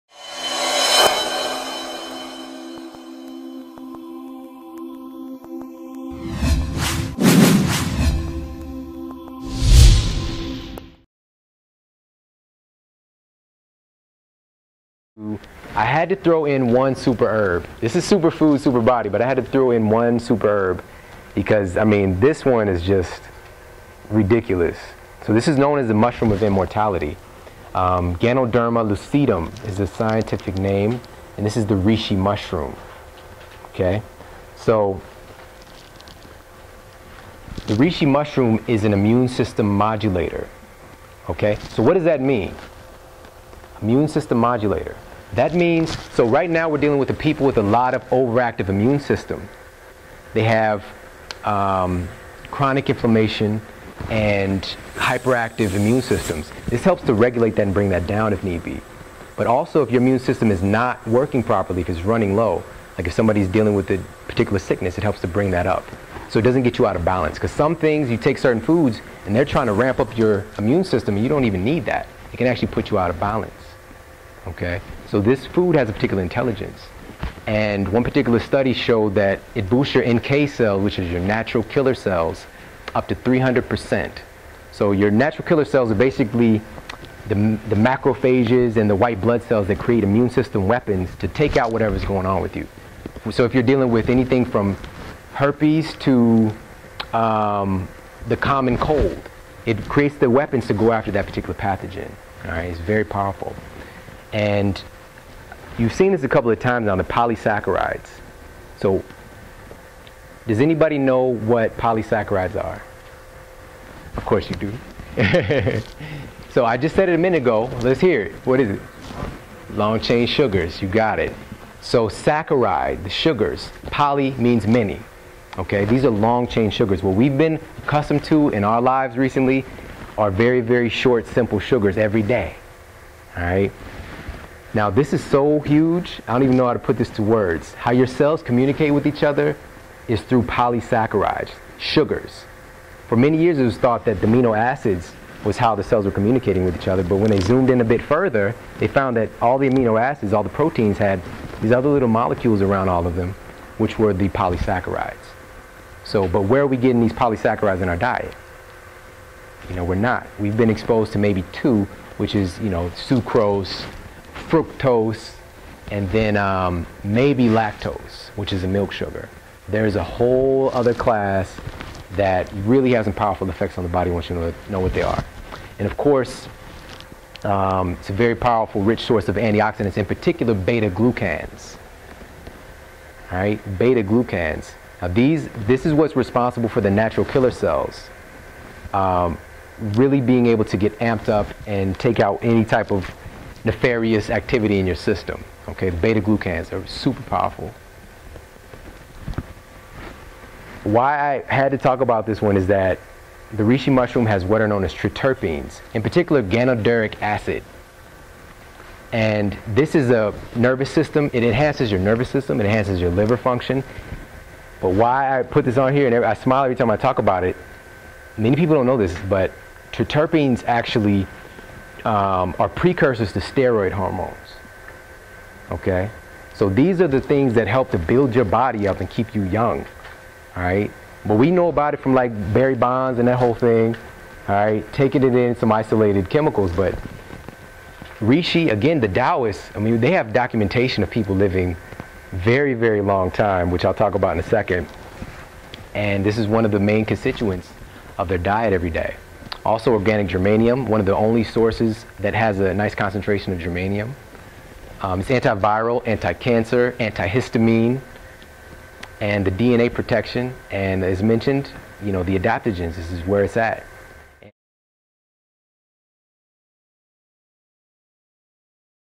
Reishi: The Mushroom of Immortality | Shawn Stevenson
The benefits of Reishi Mushroom (also known as Ling Zhi) have been prized for over 5000 years.  Reishi provides a rich source of beta-glucans and polysaccharides.

Reishi is a tonic herb that cultivates spiritual energy, modulates the immune system, and promotes longevity.